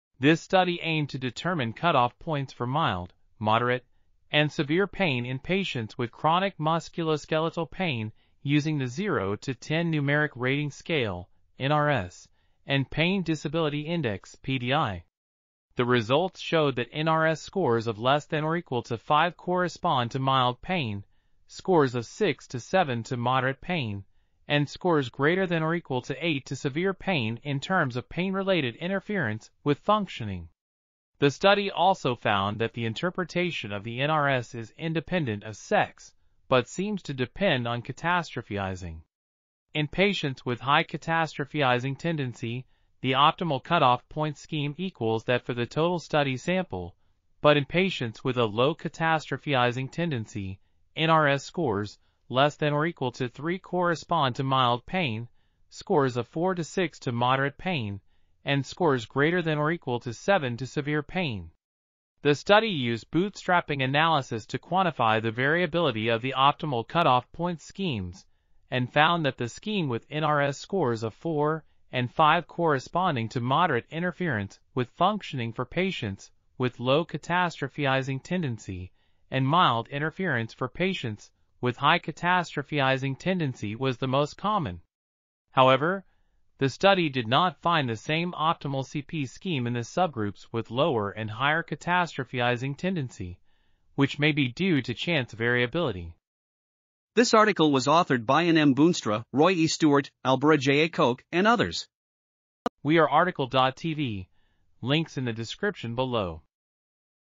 Cut-off points for mild, moderate and severe pain on the numeric rating scale for pai... | RTCL.TV
### Keywords ###

### Article Attribution ###
Title: Cut-off points for mild, moderate and severe pain on the numeric rating scale for pain in patients with chronic musculoskeletal pain: variability and influence of sex and catastrophizing.
Authors: Anne M. Boonstra, Roy E. Stewart, Albère J.A. Koke, René F.A. Oosterwijk, Jeannette L. Swaan, Karlein M.G. Schreurs ,and Henrica R. Schiphorst Preuper

### Video Timestamps ###
0:00:00 - Summary
0:01:36 - Title
0:01:42 - Tail
0:01:47 - End